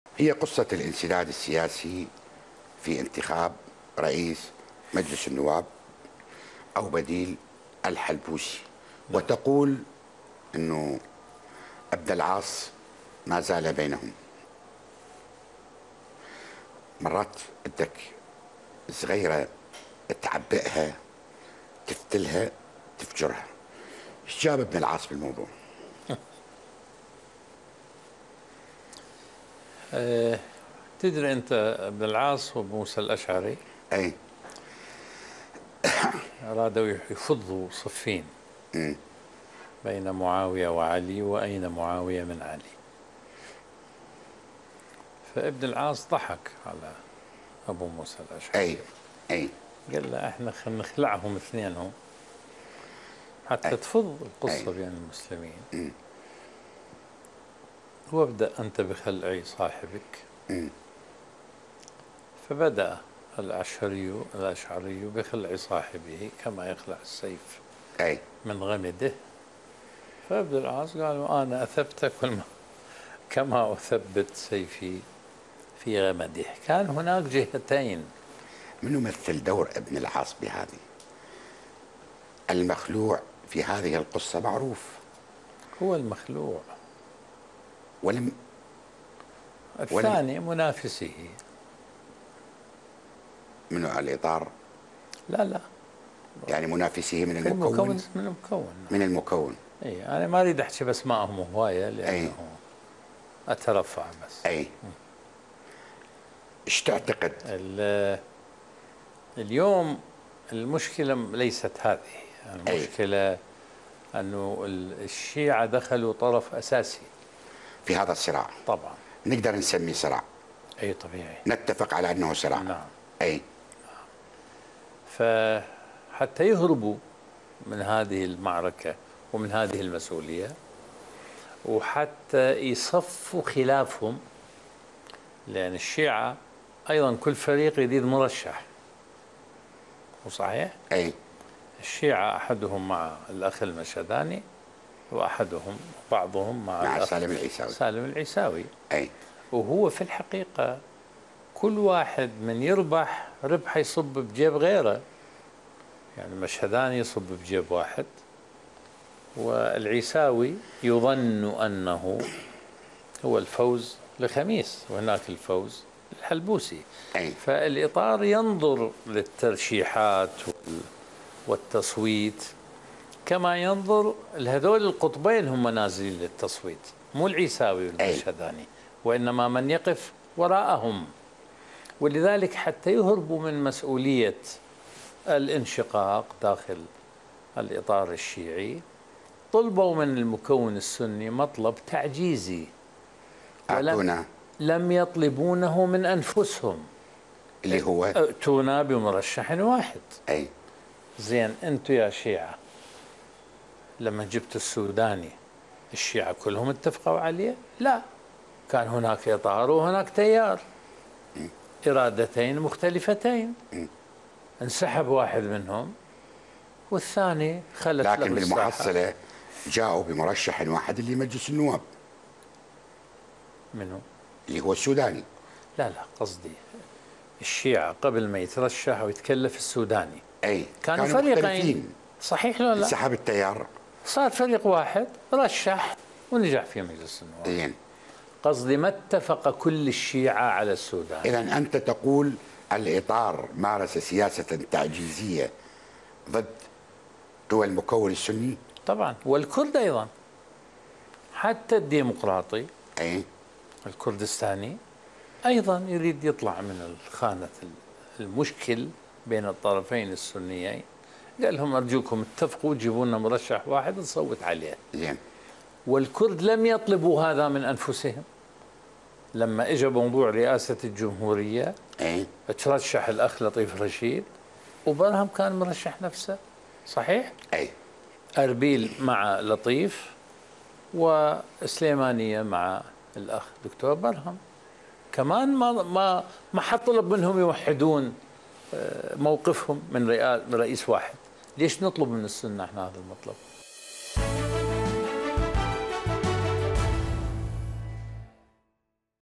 الشابندر يكشف لغز ابن العاص واختيار رئيس جديد للبرلمان العراقي
موقع قناة التغيير الفضائية  الالكتروني